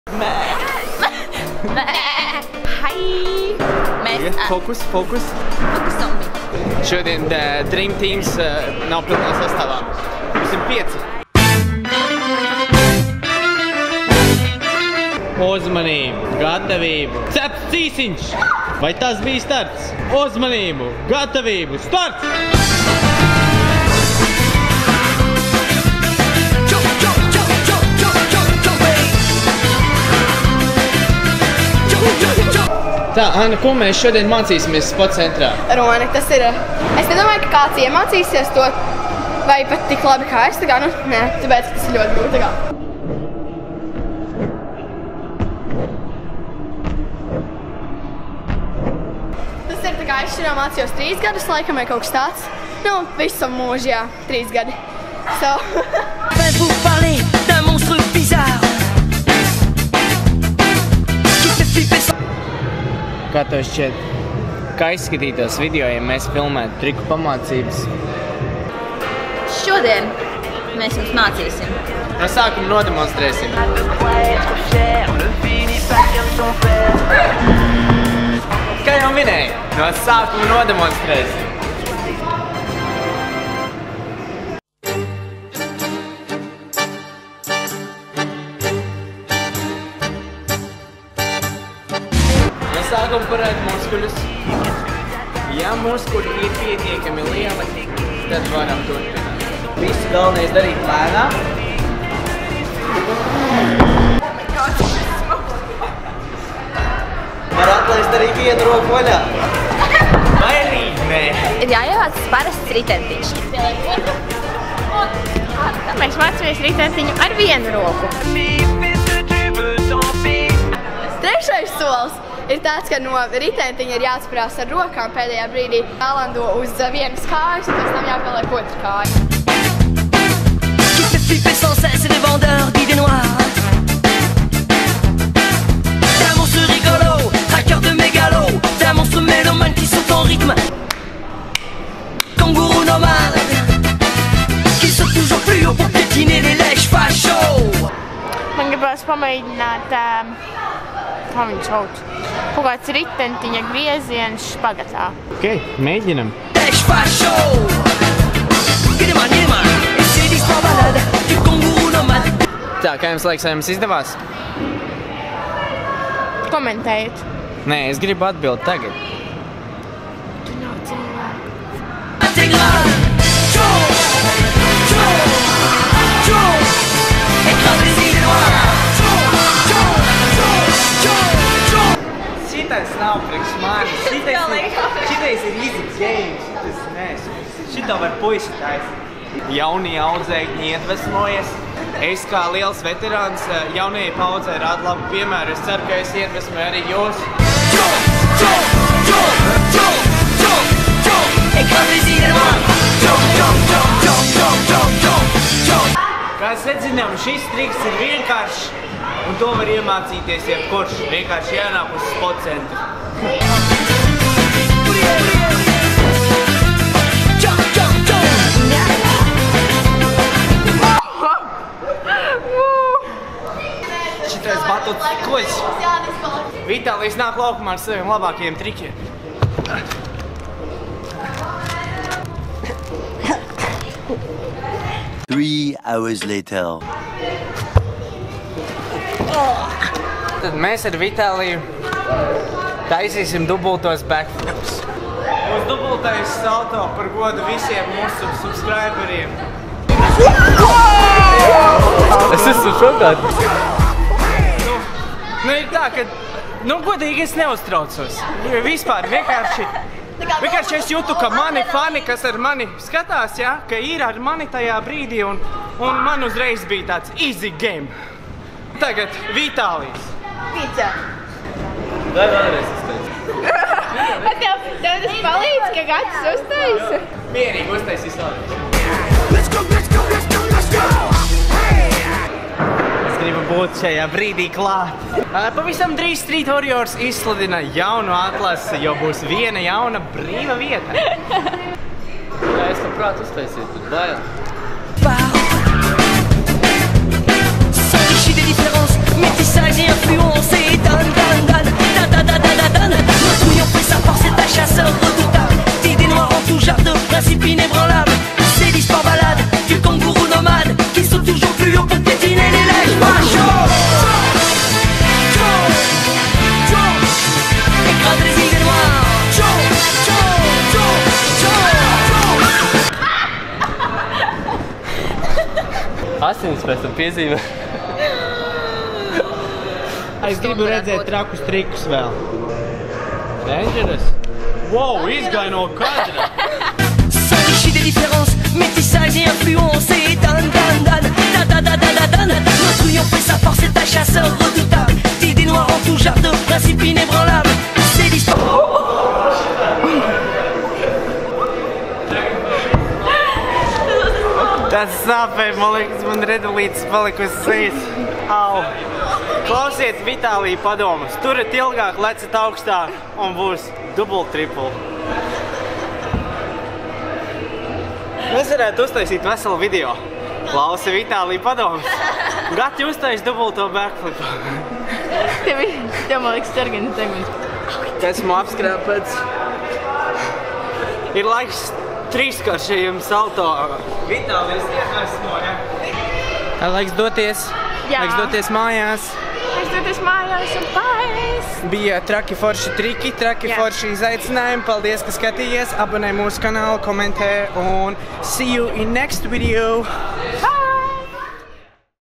SKATIES & MĀCIES - 1. SĒRIJA
Šajā vlogā devāmies uz The Spot Center, lai jums visiem iemācītu jaunus trikus. Skaties un mācies!